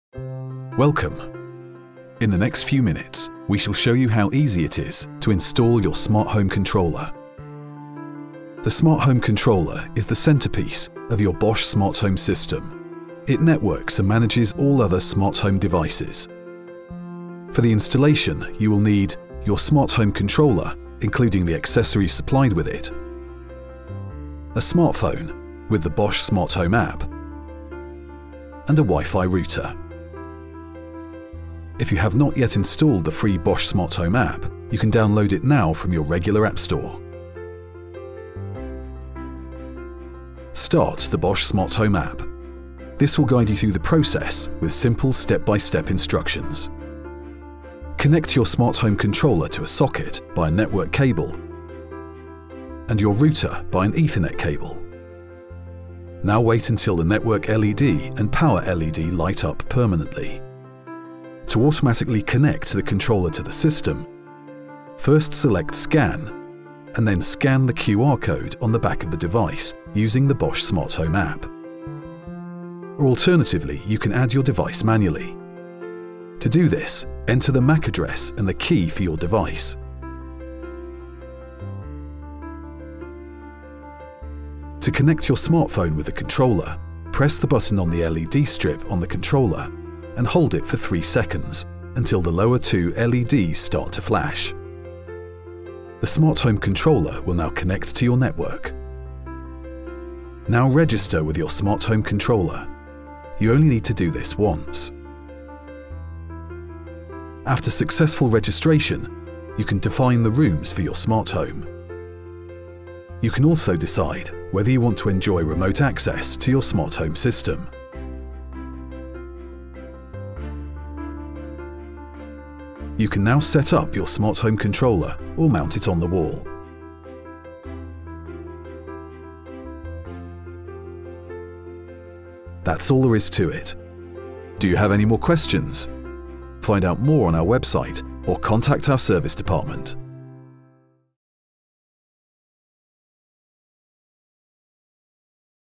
Installation: Smart Home Controller | Bosch Smart Home
This tutorial explains how to install the Bosch Smart Home Controller. The controller is the smart nerve centre of your Bosch Smart Home and connects all your System Solution products to the Bosch Smart Home app. For the installation you require the controller with the delivered supplies, your smartphone with the Bosch Smart Home App and an internet router, along with an Ethernet cable.

If you have any questions about our product, please contact our service department: